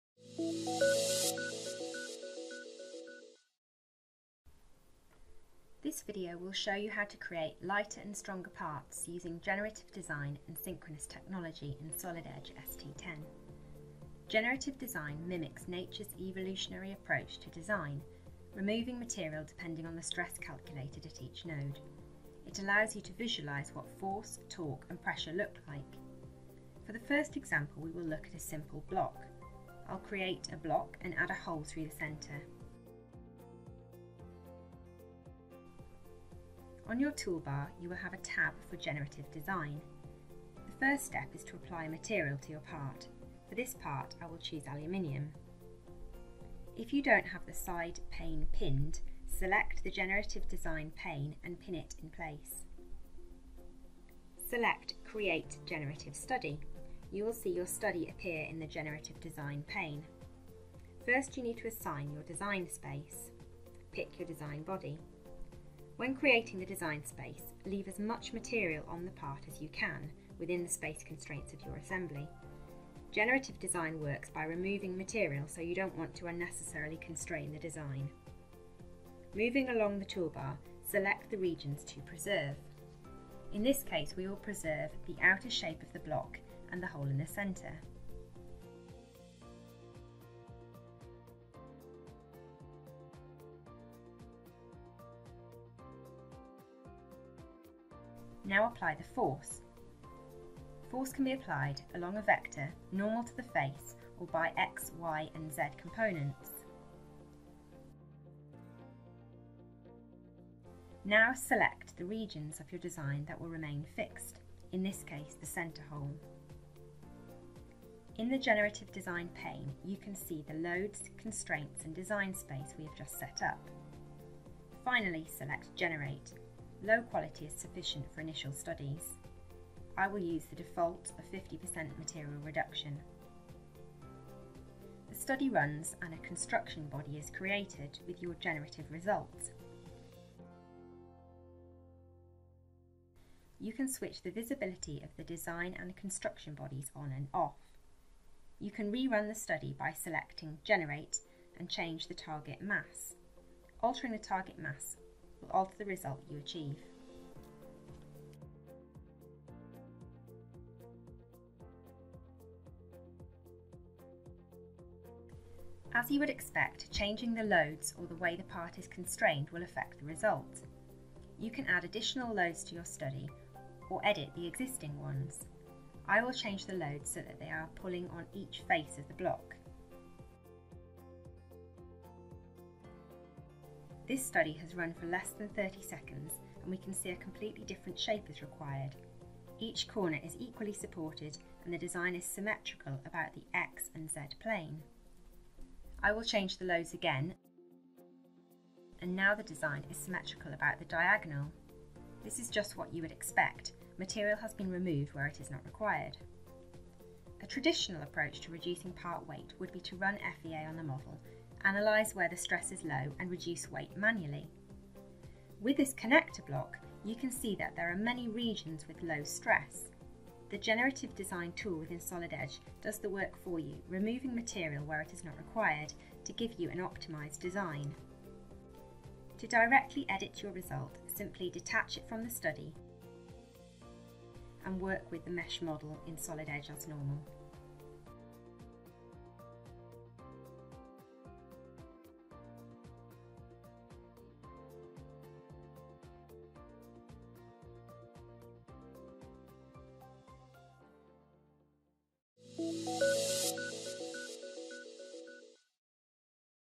Solid Edge ST10 - Getting started with Generative Design - FREE 30 Day Trial
The world of 3D CAD technology has changed, discover Solid Edge Generative Design with our useful user guide. Request your FREE 30 Solid Edge trial